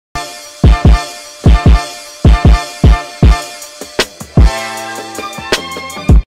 Roasted Quail with Fennel Mustard Cream | Fossil Farms' Kitchen
This mouthwatering creation is a Roasted Quail with Fennel Mustard Cream, paired with a vibrant Rainbow Cherry Tomatoes blend and Artichoke. 😋